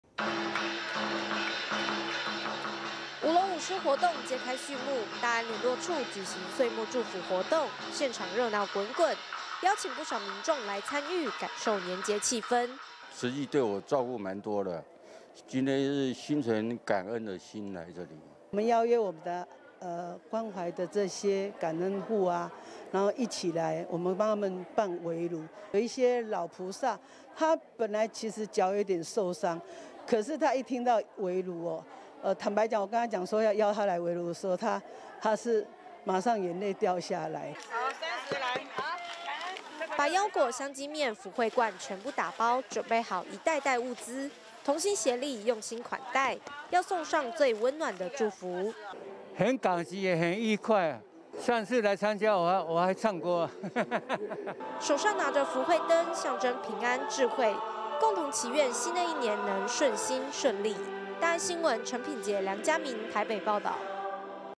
大安聯絡處歲末祝福 熱鬧圍爐好運到
這陣子，慈濟在各社區舉辦歲末祝福，感恩過去一年的平安，祈禱今年也是平安的一年。台北市的慈濟大安聯絡處志工，還邀請53戶照顧戶、關懷戶一起參與，除了歲末祝福，還有冬令發放與圍爐，提前歡度新年。

舞龍舞獅活動揭開序幕，大安聯絡處舉行歲末祝福活動，現場熱鬧滾滾，邀請不少民眾來參與，感受年節氣氛。

關懷戶 劉先生：「慈濟對我照顧滿多的，今天是心存感恩的心來這裡。」

慈濟志工 謝碧玲：「邀約我們的關懷的這些關懷戶，然後一起來，我們幫他們辦圍爐，有一些老菩薩，他本來腳有點受傷，可是他一聽到圍爐，坦白講，我跟他講要邀他來圍爐的時候，他是馬上眼淚掉下來。」

把腰果、香積麵、福慧罐全部打包，早在前一天就準備好一袋袋物資，同心協力，用心款待，要送上最溫暖的祝福。

關懷戶 李先生：「很感激也很愉快，上次來參加，我還唱歌。」

手上拿著福慧燈，象徵平安、智慧，共同祈願，新的一年能順心、順利。

陳品潔 梁家銘 台北報導